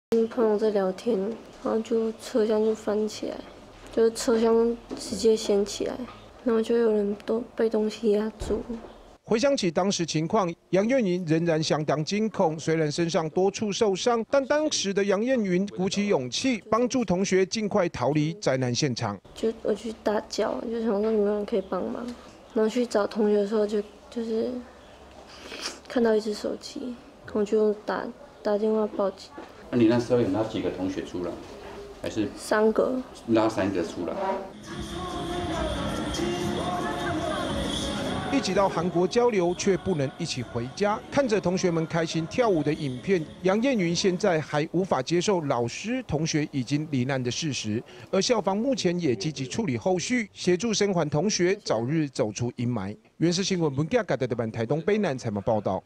逃過翻車死劫 陽宴筠幫助同學逃離事故現場 2018-10-23 IPCF-TITV 原文會 原視新聞
回想起當時情況，陽宴筠仍然相當驚恐，雖然身上多處受傷，但當時陽宴筠鼓起勇氣，幫助同學盡快逃離災難現場。

一起到韓國交流，卻不能一起回家，看著同學們一起開心跳舞的影片，陽宴筠現在還無法接受老師、同學已經罹難的事實，而校方目前也積極處理後續，協助生還同學，早日走出陰霾。